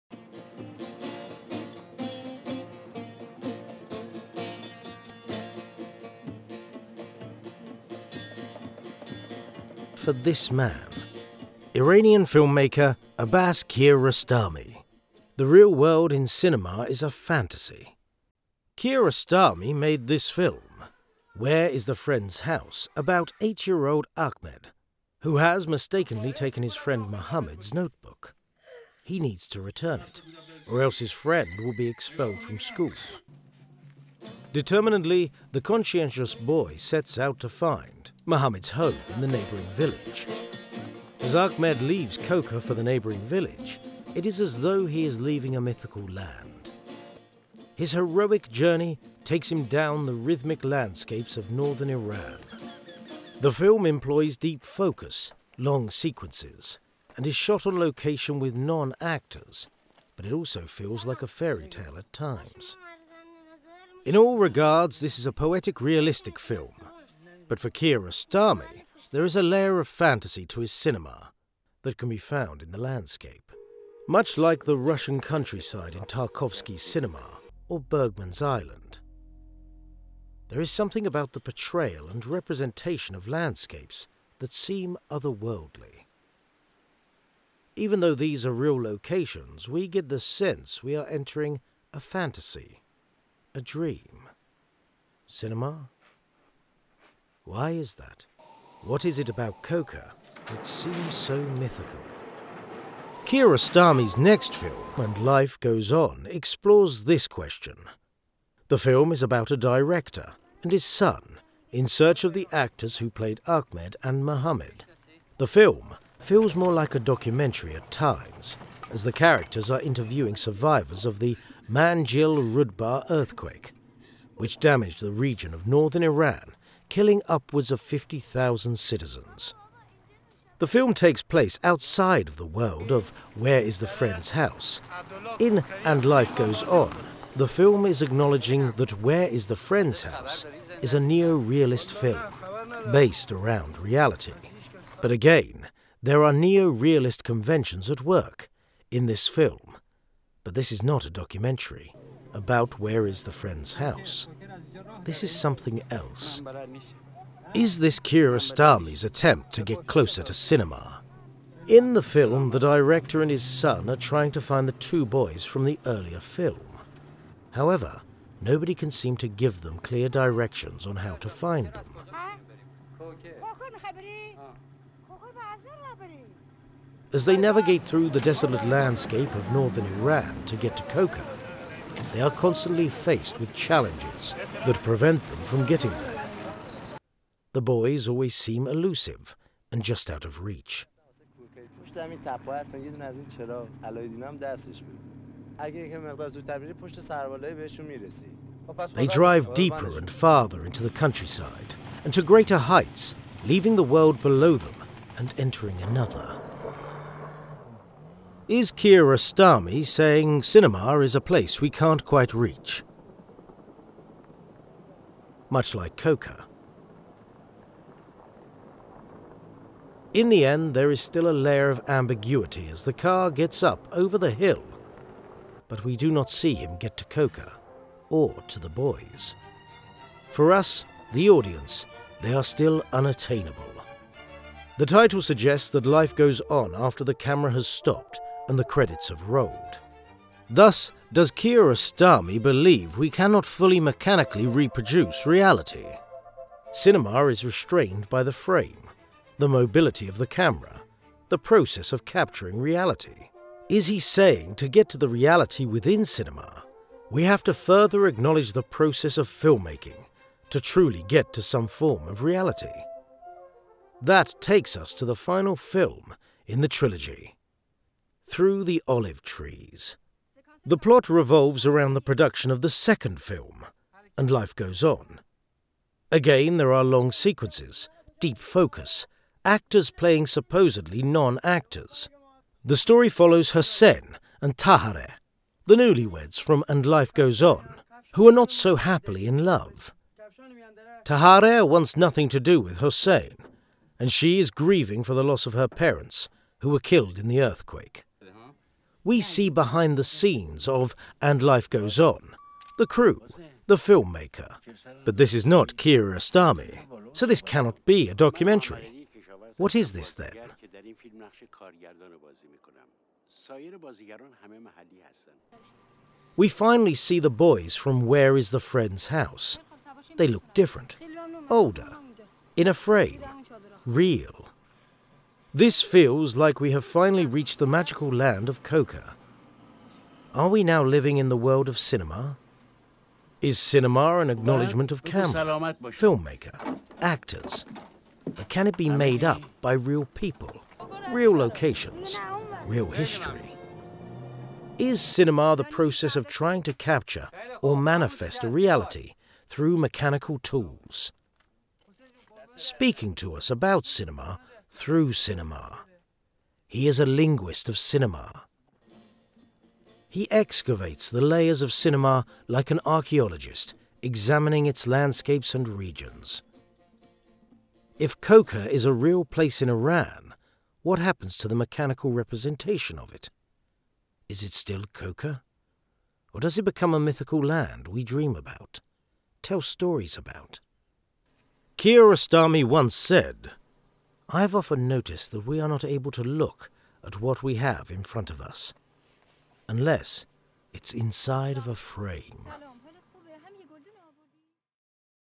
The Koker Trilogy: The Fictional, The Real, & The Process
This episode we examine Abbas Kiarostami's Koker trilogy which explores the ontology of cinema through the blending of the fictional, the real, and the process of creating and capturing cinema.